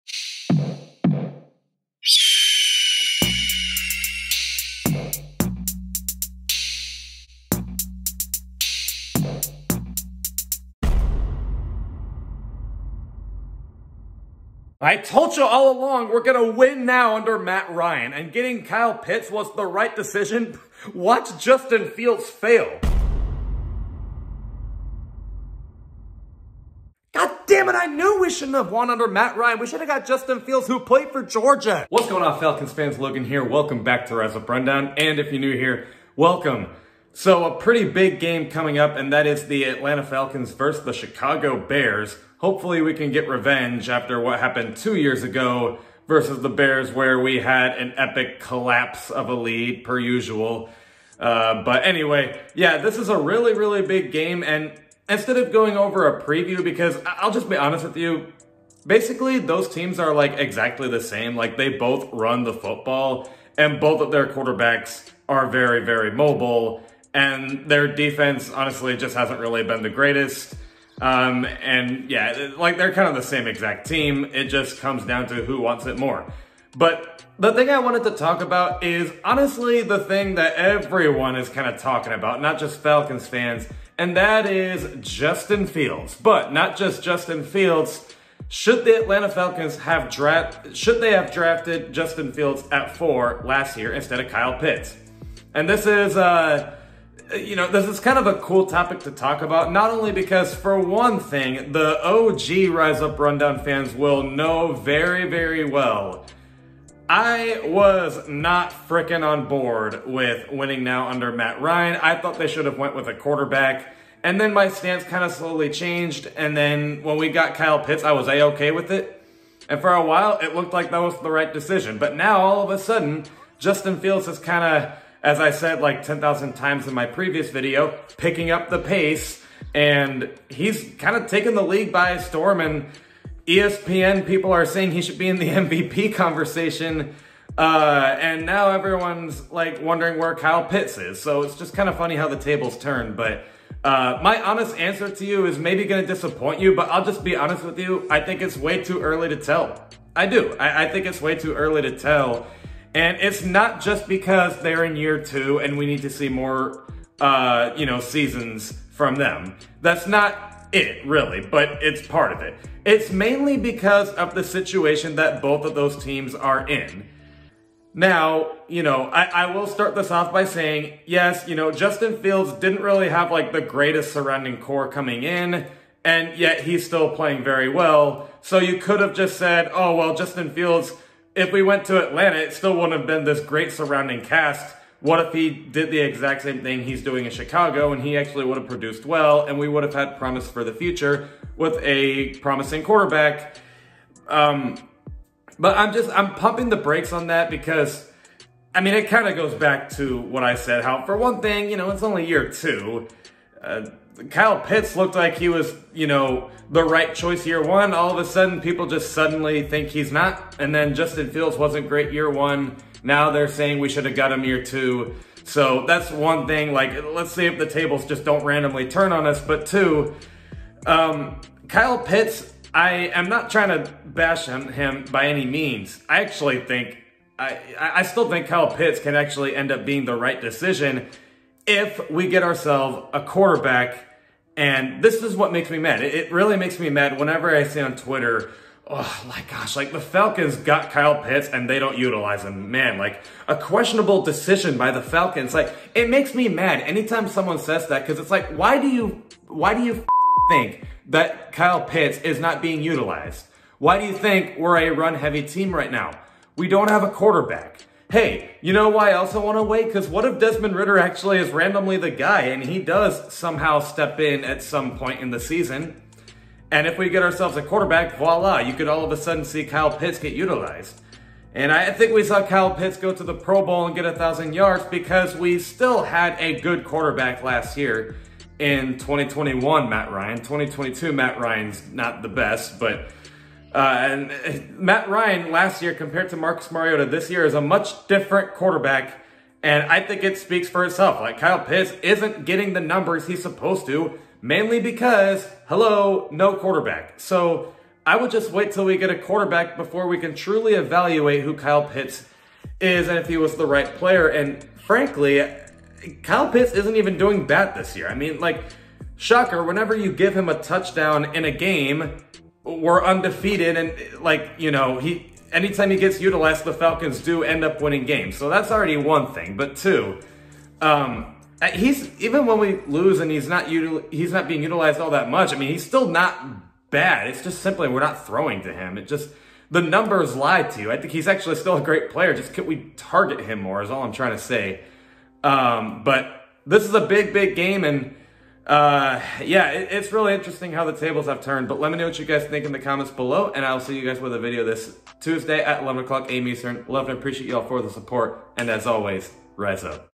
Should The Falcons Have Drafted Justin Fields Over Kyle Pitts? | Rise Up Rundown
Uh oh! Falcons fans are back at it again in making heated debates over the 2021 NFL draft…. A YEAR LATER!! This time, Falcons fans are going back and forth on if drafting Kyle Pitts over Justin Fields was the right call! What is my opinion on that? In this video, we go over just that!